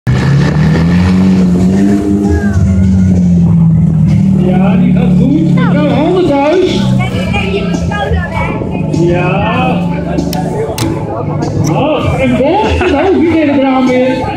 Monster Mania Uithoorn 2014 - 06 - Op 2 Wielen (3)
Monster Mania 2014 in Uithoorn.

De gebruikelijke kunstjes met auto's. Nu rijdt er iemand uit het publiek mee in de auto.